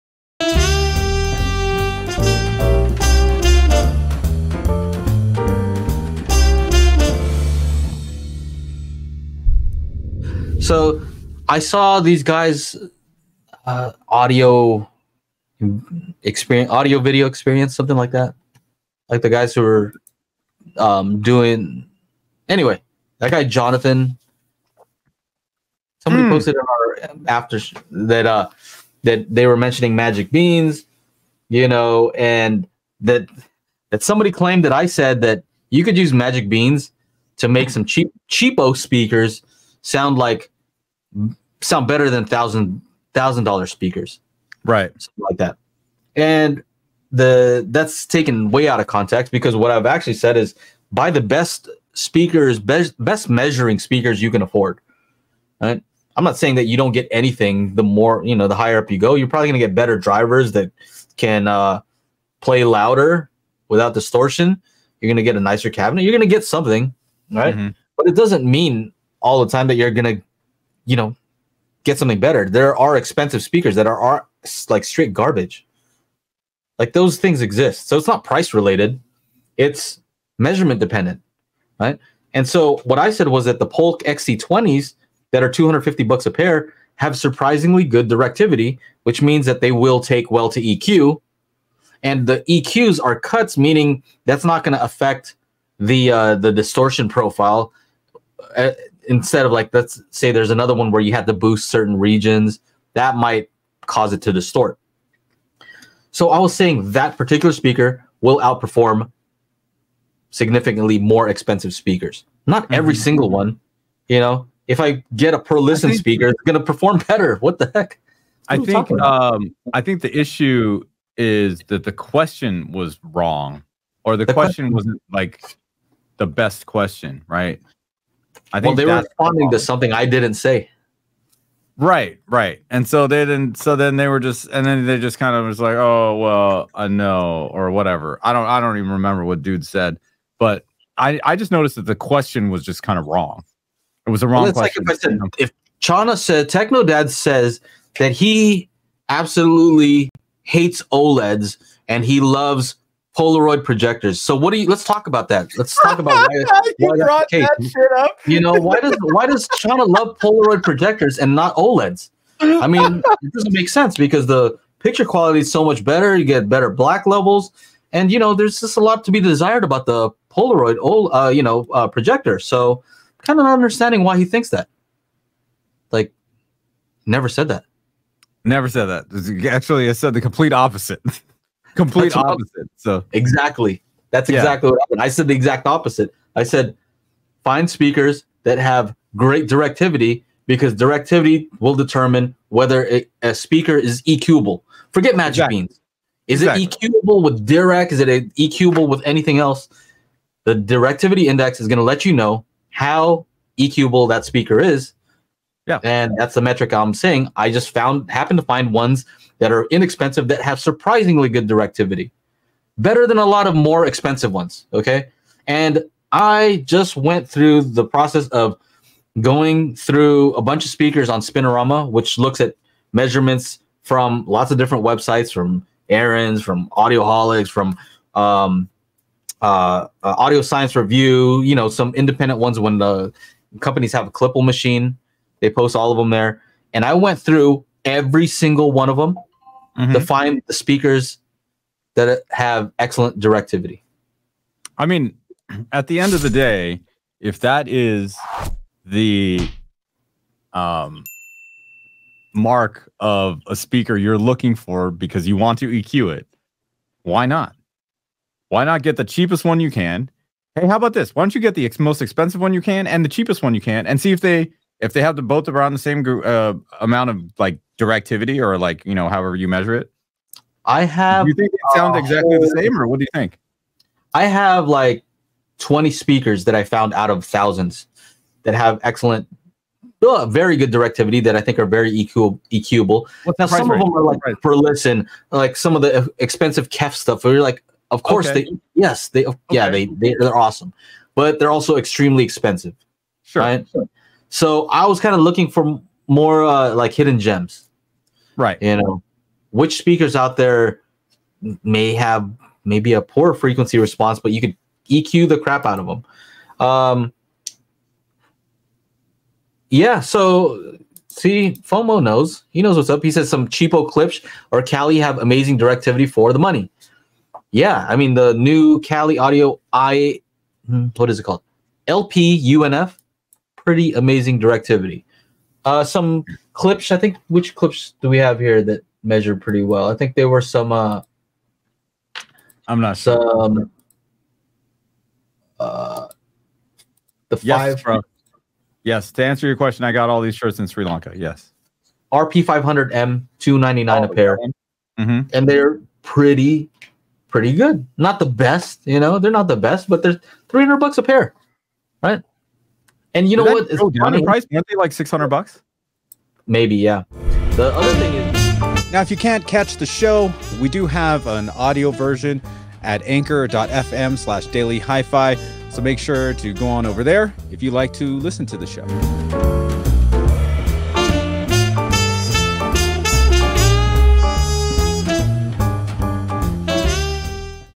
Importance Of: On Axis Frequency Response And Directivity When Buying Speakers?
Make sure you are asking the right questions. What is the most important speaker measurement when it comes to home theater and EQ? Directivity! Do you think otherwise? What do you look for in a speaker measurement regimen? What would you guys to have clarified, maybe from Erin himself? Or if you have any other questions just let us know!